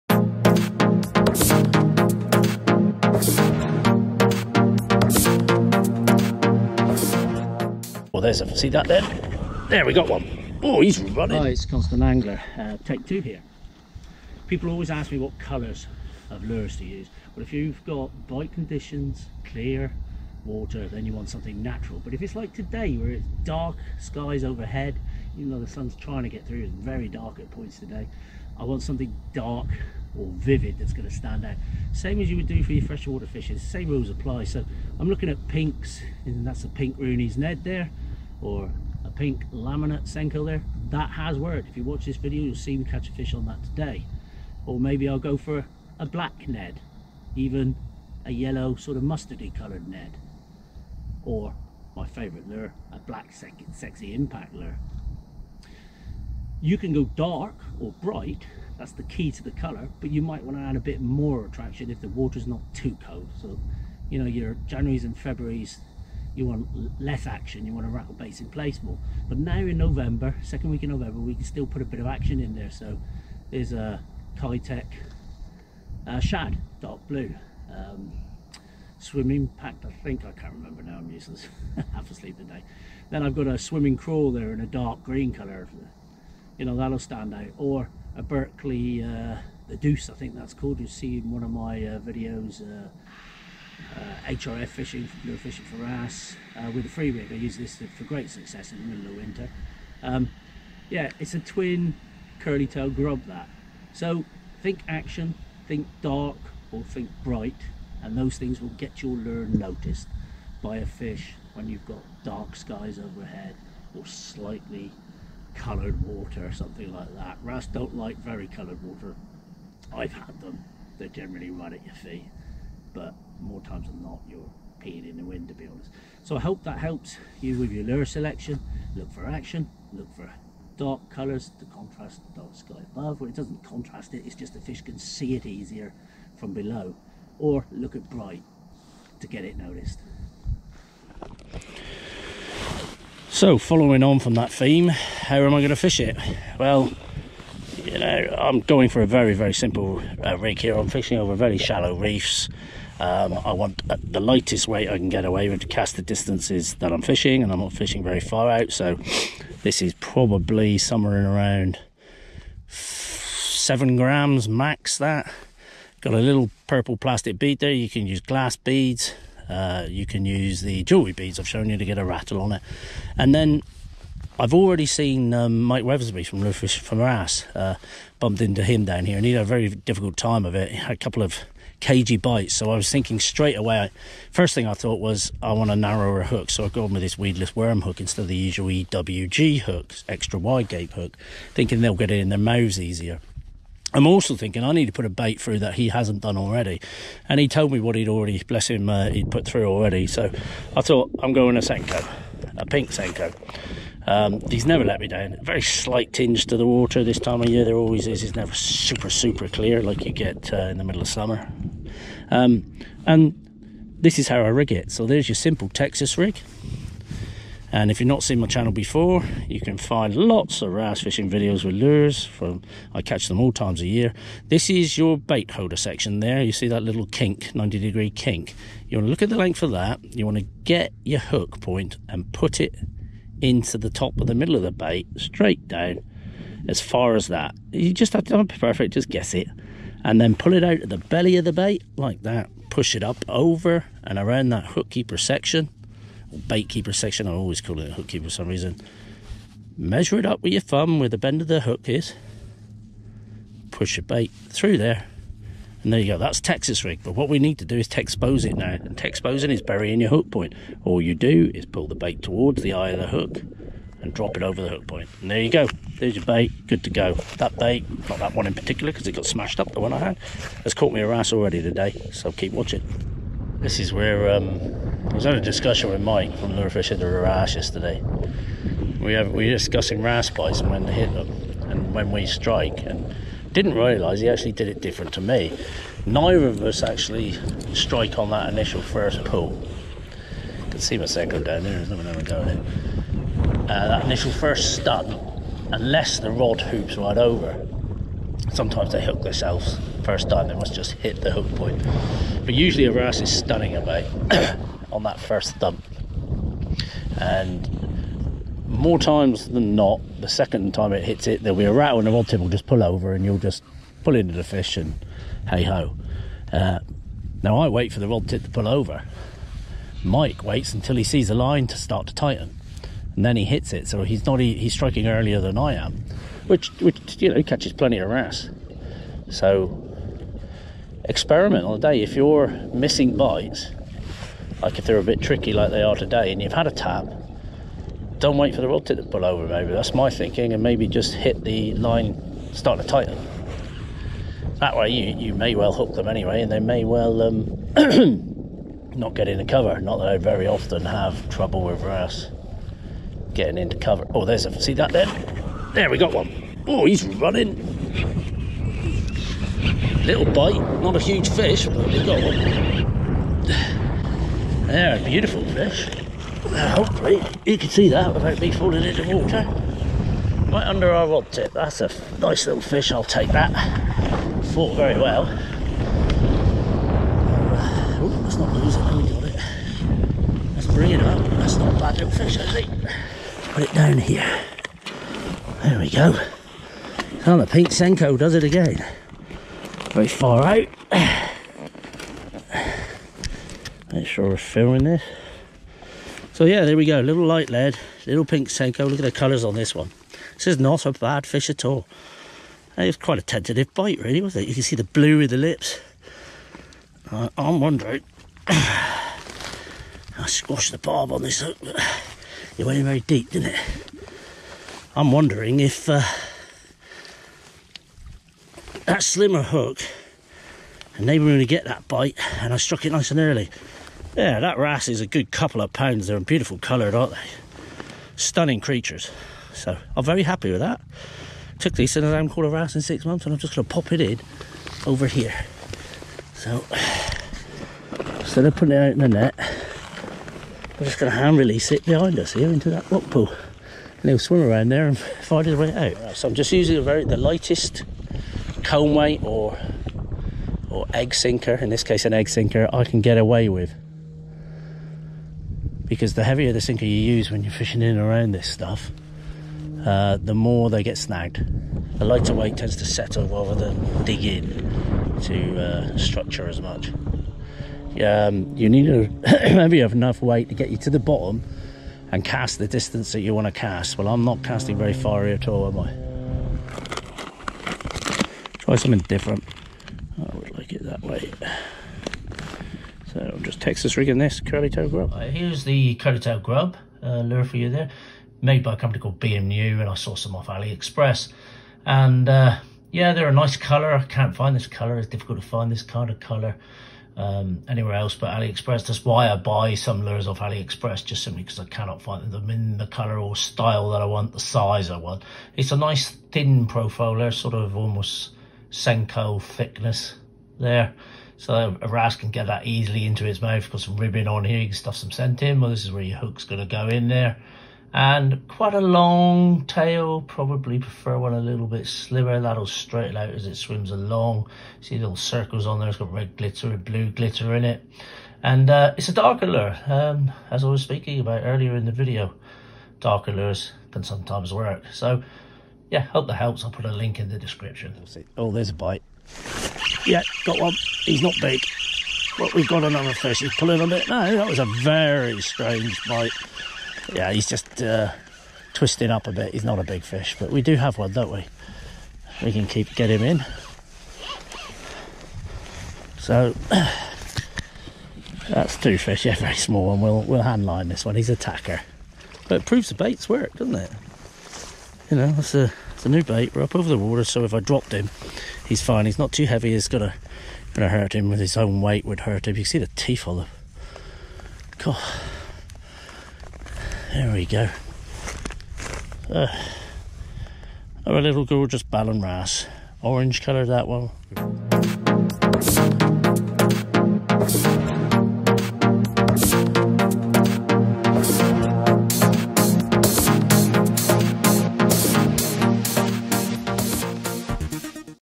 Lure Fishing for Wrasse - Choosing Lure Colour - HRF
Wrasse Fishing with Lures - Choosing Lure Colour - HRF
Choosing the correct lure colour to match the conditions on the day can make or break your session. Here i will advise you on what works for me. I also touch on lure action and how it can get your lure noticed.
Hard Rock Fishing is a fast growing branch of our sport so why not give it a go. Wrasse fight very hard as you can see from the fish i catch in this video.
00:00 Wrasse Fishing with Lures - Choosing Lure Colour
00:10 How to choose the correct colour lure on the day
02:22 Rig for the day: The Texas Rig and how i adapted it to the type of Wrasse bite on the day
04:33 How to strike a Wrasse bite
05:51 Fish on! First Wrasse hooked
07:25 Choosing the correct weight
07:57 Change of lure
08:05 New Lure for my Wrasse Fishing
08:58 First Wrasse to the new lure

B & U Curly Tail Lures
Sexy Impact Pin Tail Lure
Keitech Swing Impact 4#:
Braid: KastKing SuperPower 20lb Yellow
HT0 Weedless Worm Hooks
EGG SINKERS 1/4oz